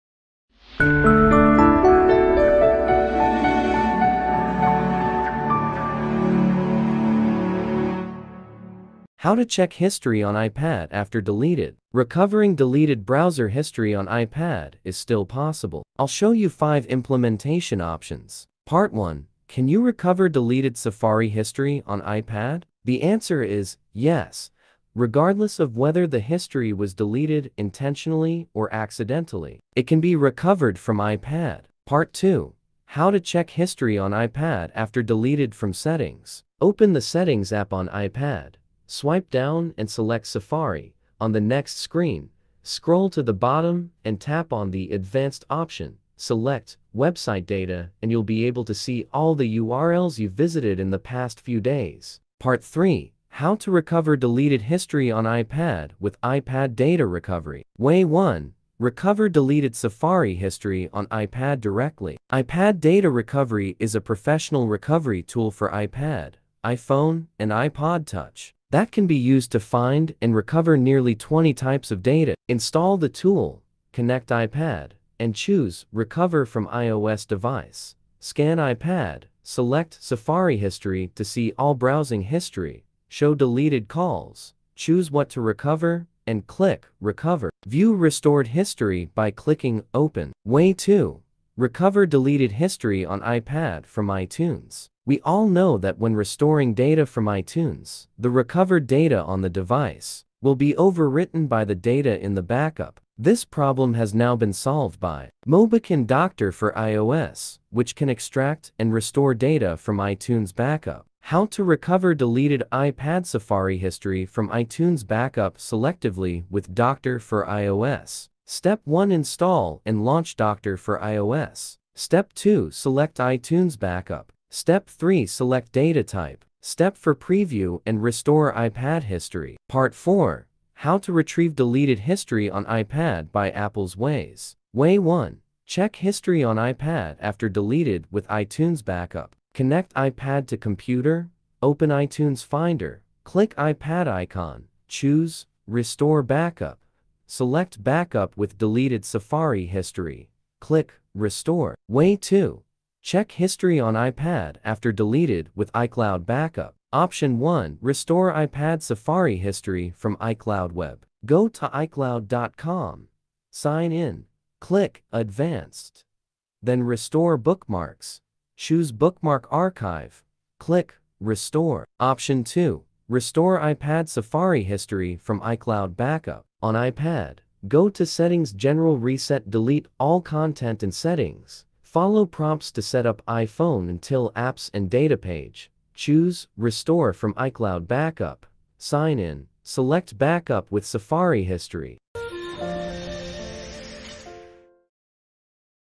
[100% Successful] How to Check History on iPad after Deleted?
How to check history on iPad after deleted? This guide offers some useful methods to recover deleted history on iPad, regardless of iPad 10/9/8 or iPad Mini/Air/Pro.

00:00 Start
00:18 Part 1: Can You Recover Deleted Safari History on iPad?
00:31 Part 2: How to Check History on iPad After Deleted from Settings
00:52 Part 3: How to Recover Deleted History on iPad with iPad Data Recovery
02:09 Part 4: How to Retrieve Deleted History on iPad by Apple's Ways